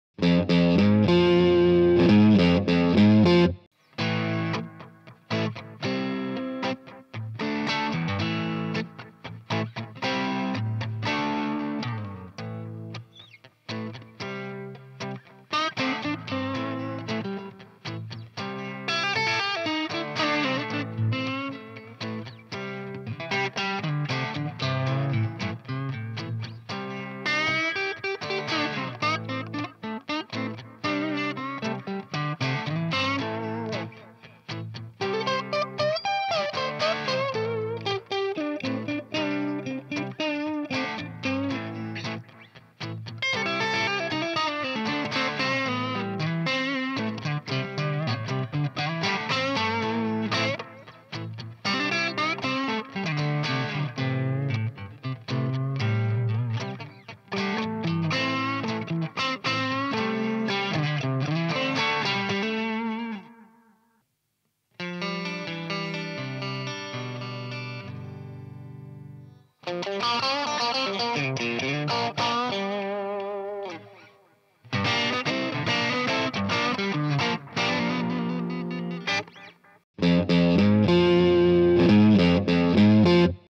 ServetteMusicTV : Fender CS Strat 62/63 Ltd JRNY Relic RW AOL
John Woolloff fait une démo de la Fender CS Strat 62/63 Ltd JRNY Relic RW AOL.